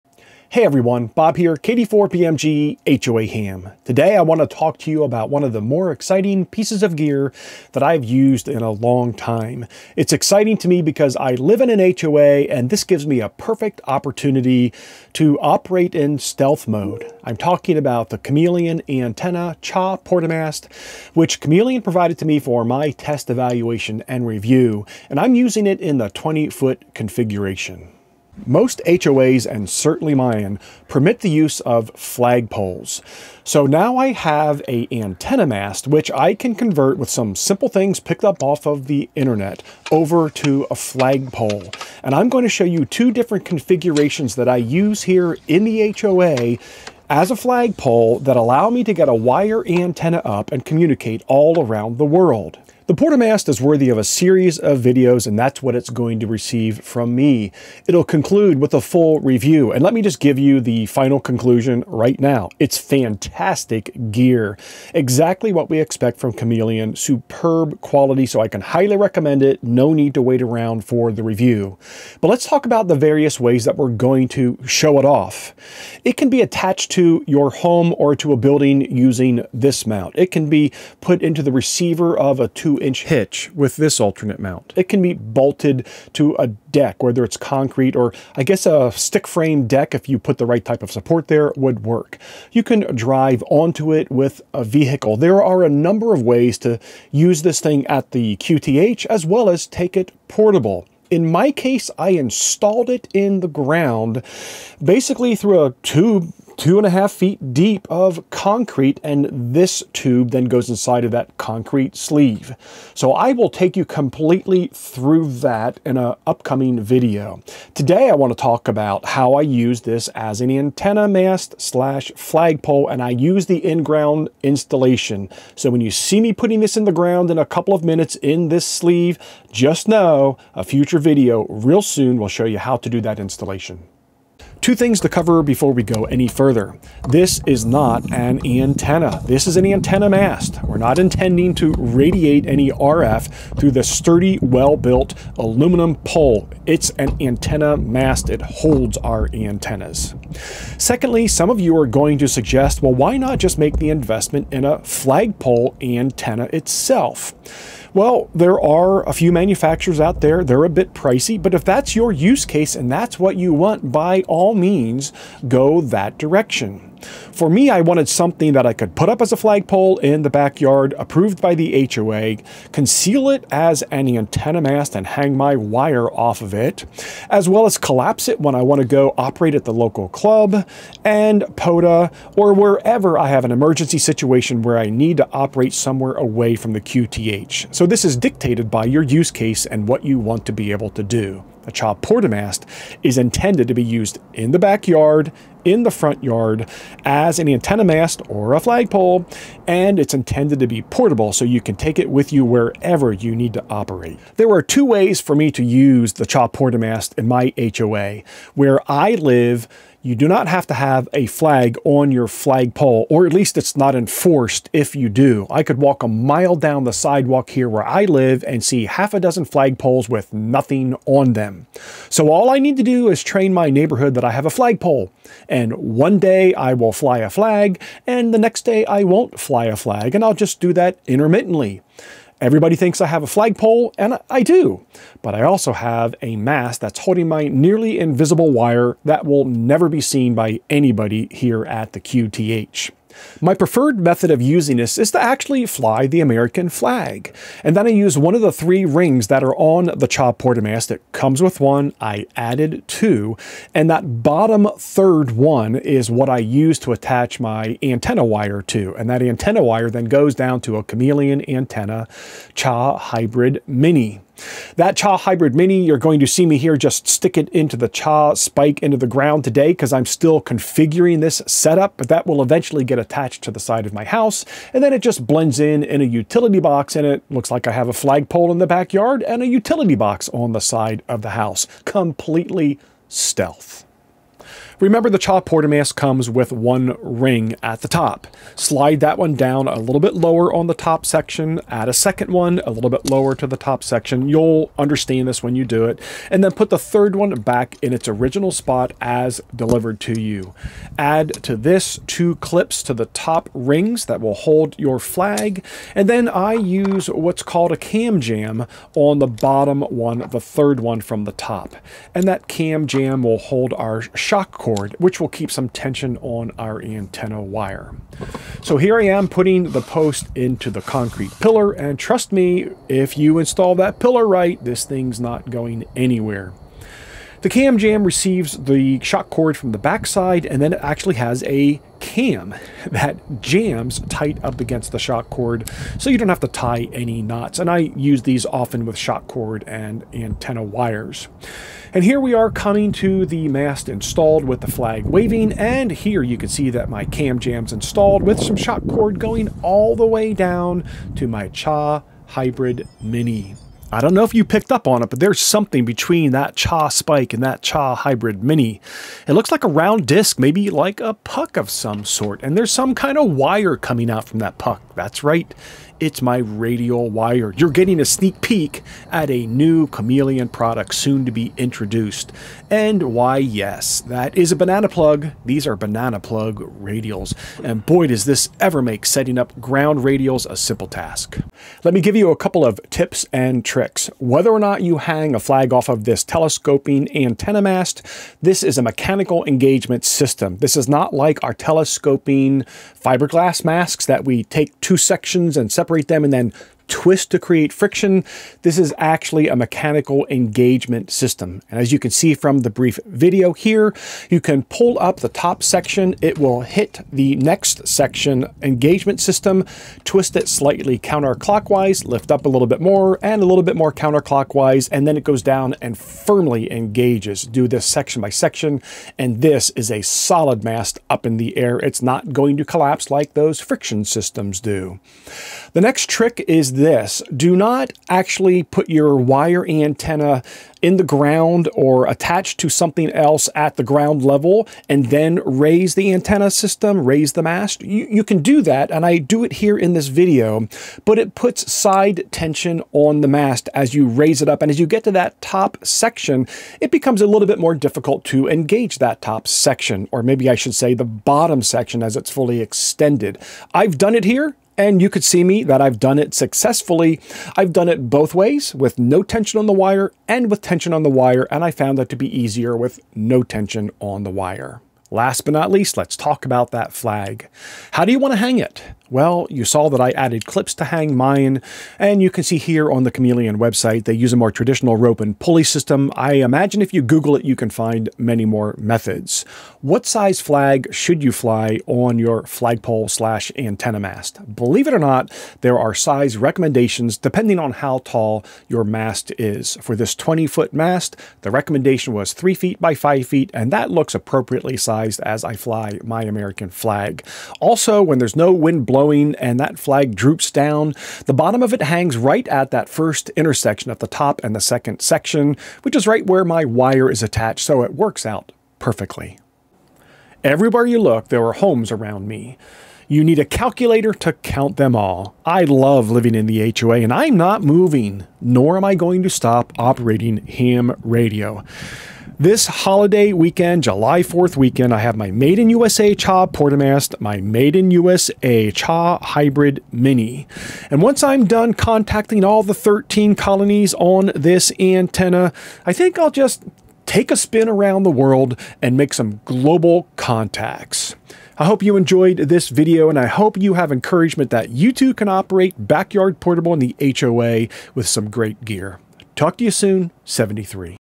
Stealth HOA Ham Radio Antenna with the Chameleon Cha Porta Mast / Flagpole - The HOA Will Never Know
This is how I operate on ham radio HF frequencies in the HOA with my stealth end fed wire supported by the Chameleon Antenna Cha Porta Mast  -- it doubles as a Flagpole

😮HOA HAM MERCH😮  A UNIQUE Badge for Every License Class

Created and owned by:
HOA Ham LLC